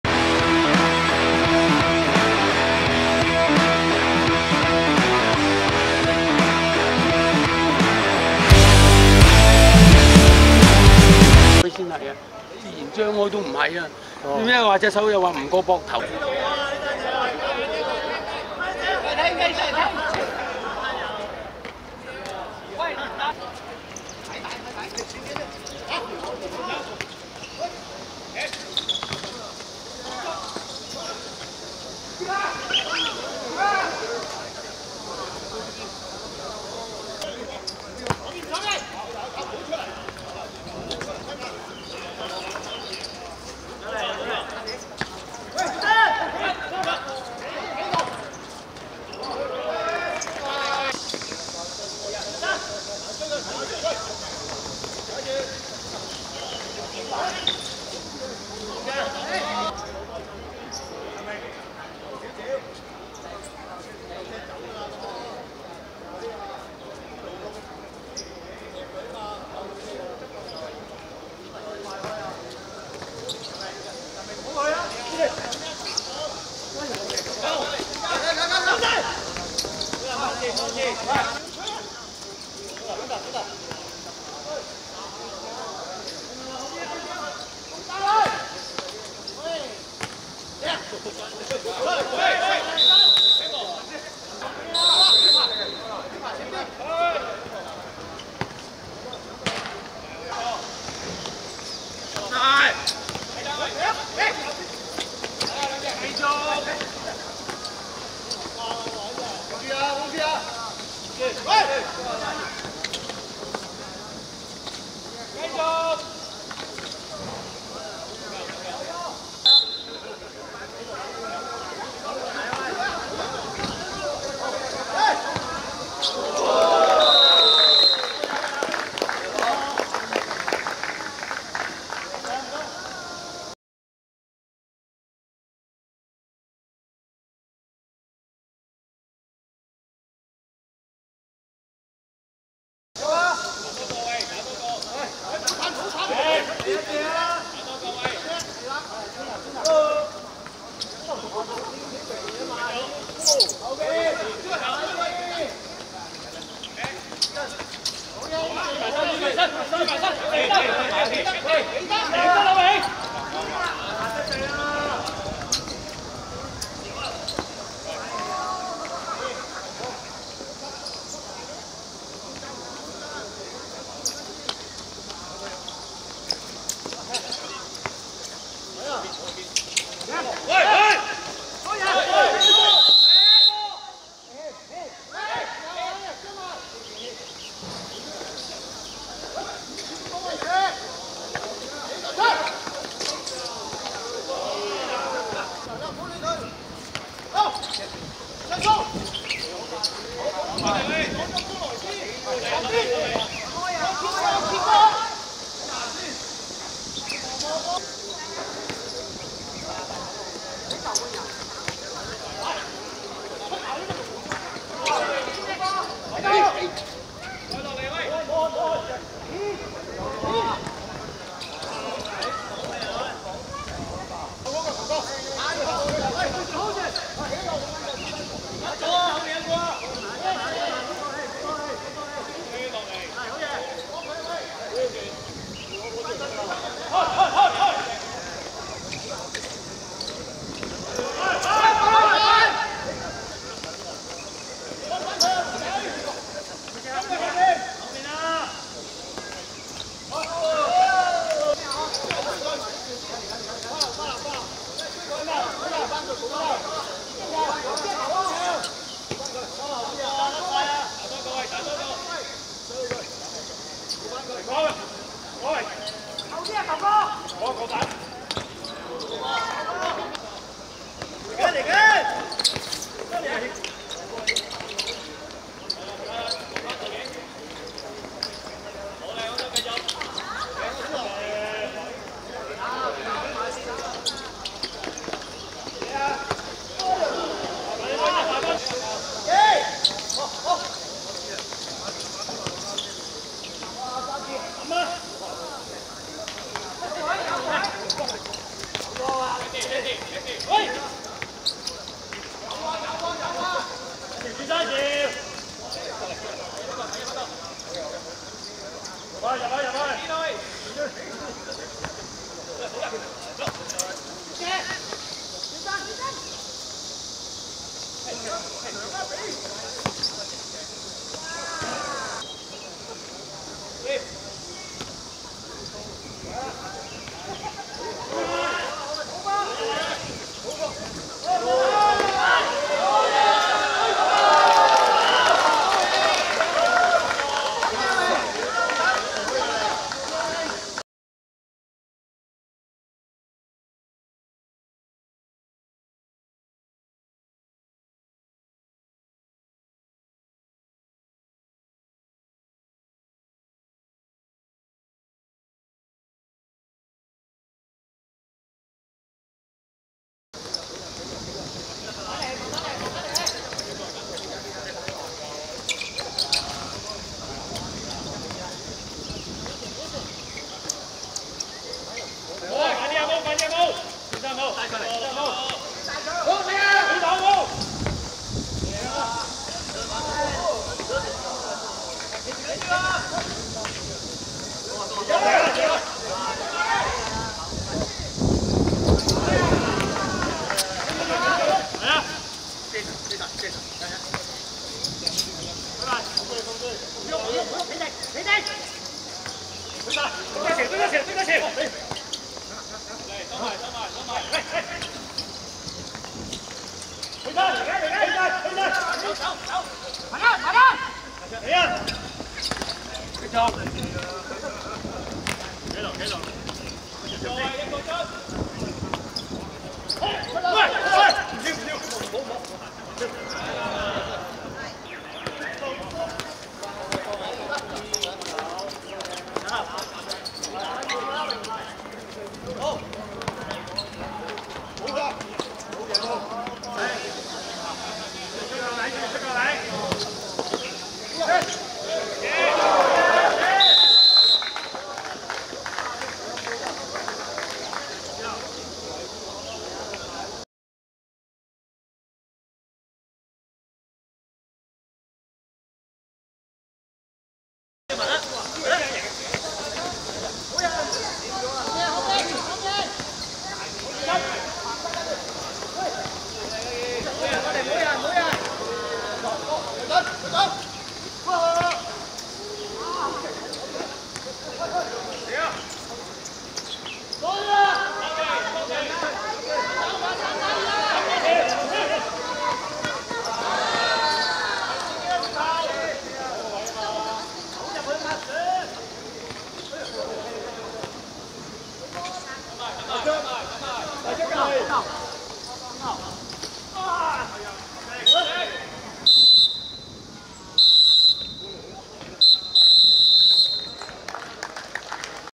榮來球聚vs奔域(2021.6.27.榮來盃足球三角賽)精華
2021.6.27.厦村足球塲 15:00
榮來球聚(橙) 2 : 1 奔域(紫)